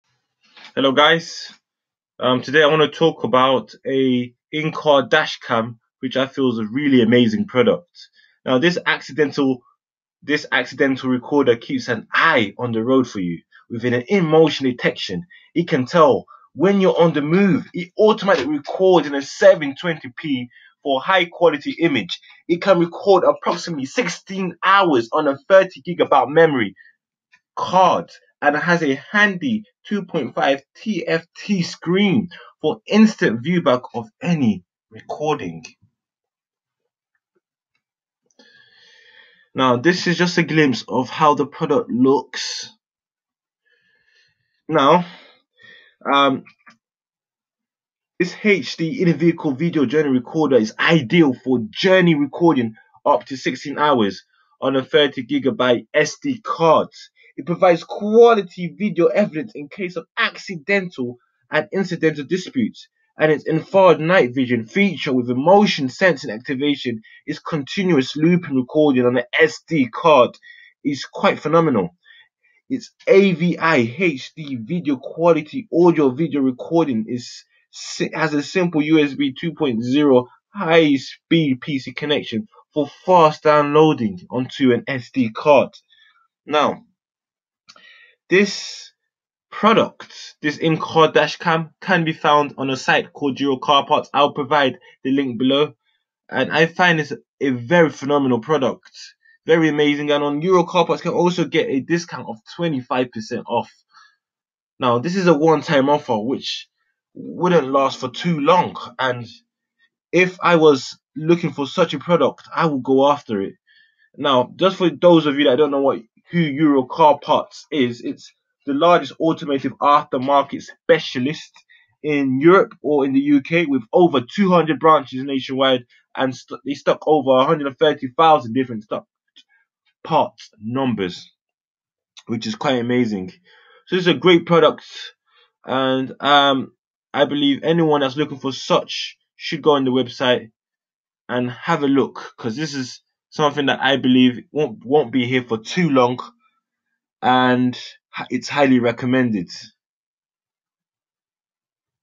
Streetwize 2.5inch Screen Compact in-car Digital Video Recorder
Our Streetwize SWREC-1 Accident Recorder keeps an eye on the road for you, With built in montion detection it can tell when you're on the move it automatically records in 720p for a high quality image. It can record upto aprox 16hours on a 32gb memory card (not included) and has a handy 2.5" TFT screen for instant viewback of any recordings.